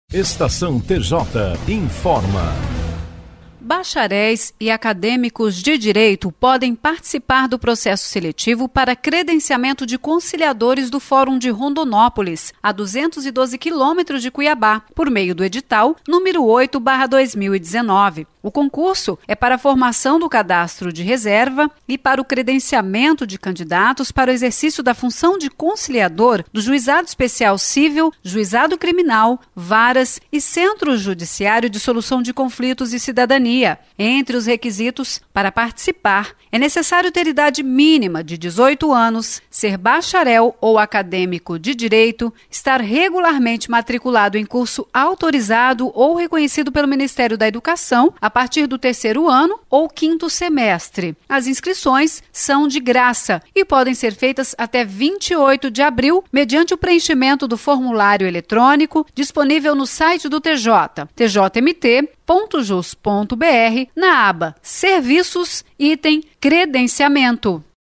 Estação TJ: Fórum de Rondonópolis abre credenciamento para conciliadores
Bacharéis e acadêmicos de Direito podem participar do processo seletivo para credenciamento de conciliadores do Fórum de Rondonópolis (212 km ao sul de Cuiabá), por meio do Edital n. 8/2019.